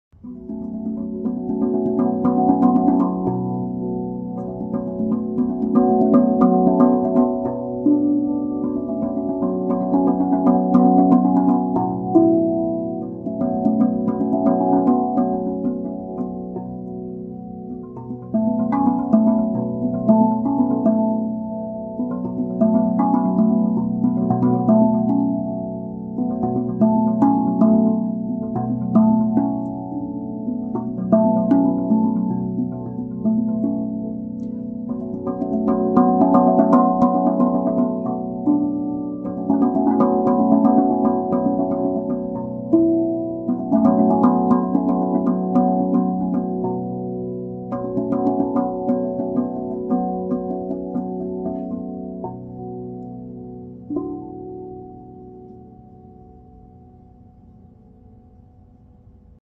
Rav (Tongue Drum) Solo
One of my adult students left me her RAV drum for a few weeks. This is a short solo with soft marimba mallets.